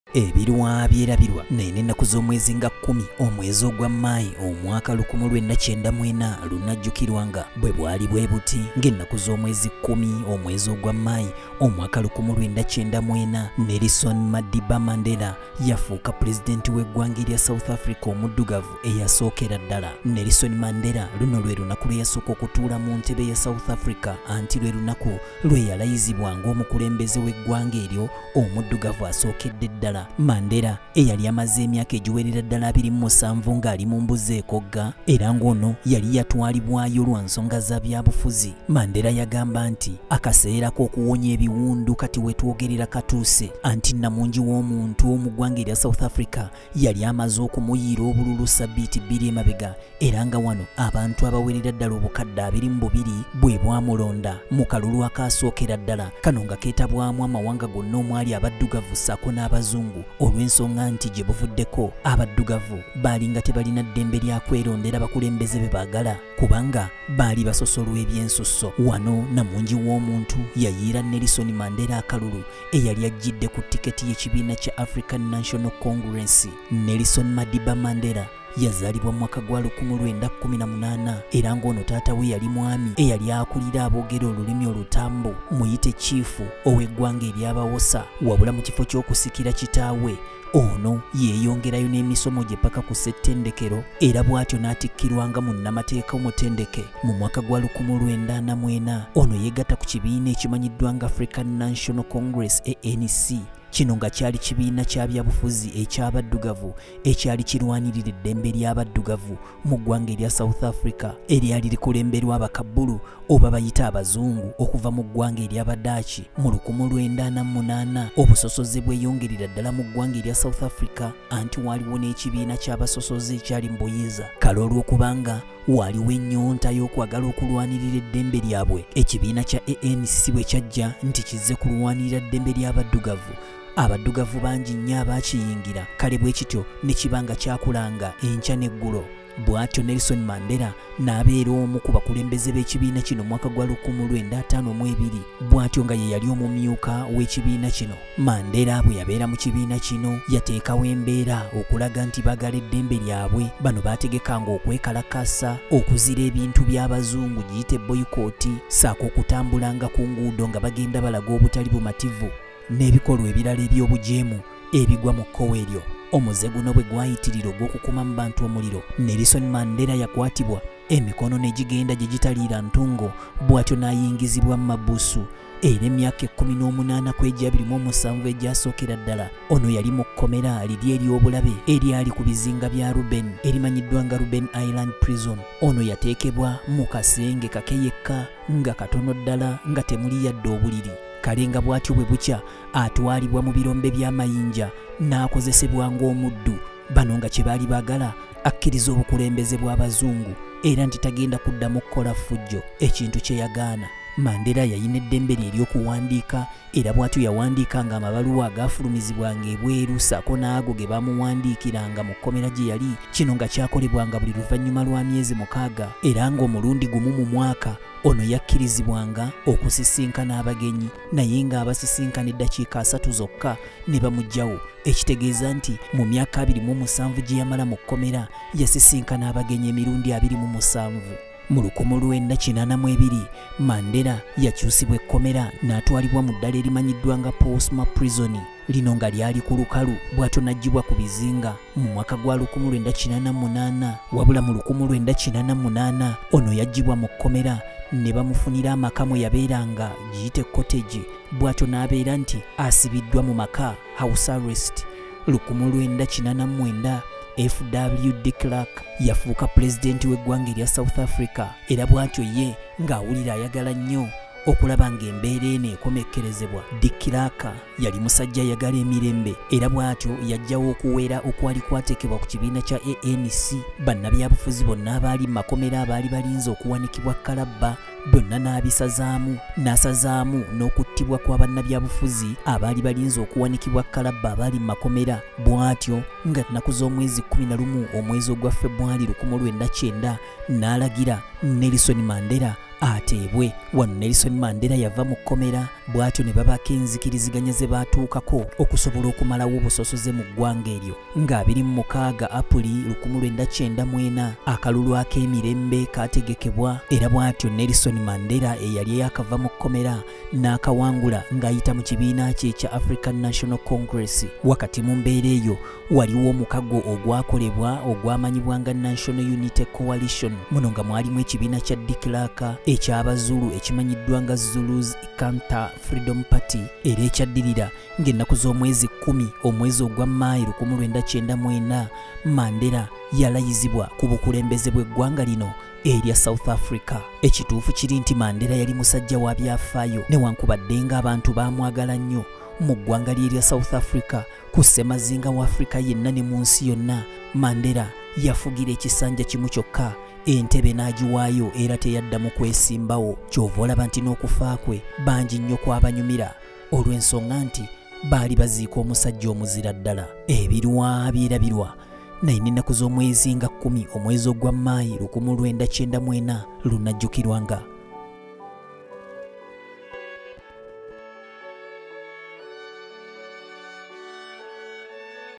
NELSON MANDERA DDALA YALI MUZIRA
YE MUDDUGAVU EYASOOKA OKUBEERA PRESIDENT WA SOUTH AFRICA